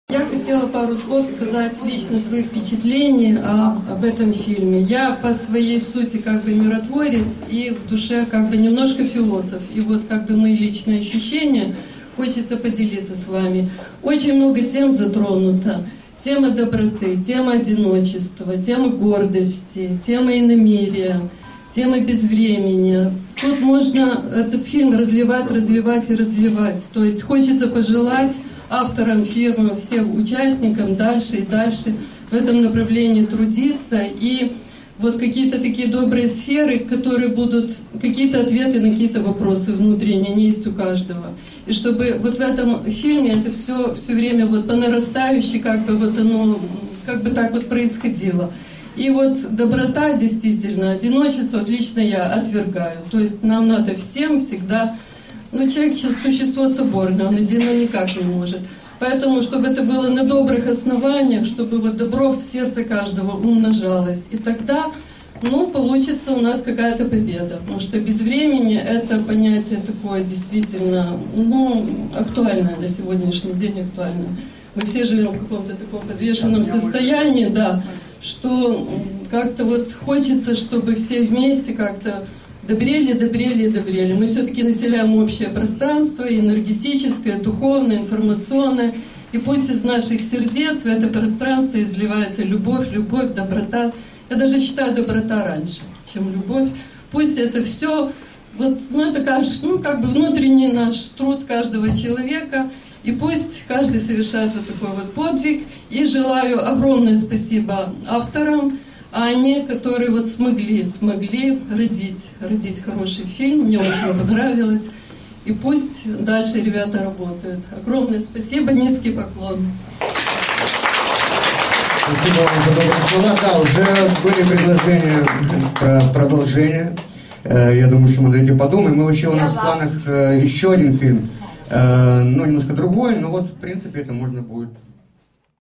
Ирина Коноваленко о фильме "БЕЗВРЕМЕНЬЕ" 21.02.2021
Презентация х.ф. "БЕЗВРЕМЕНЬЕ"
СЛУШАЙТЕ САМОЕ ОДЕССКОЕ РАДИО ПАЛЕ-РОЯЛЬ!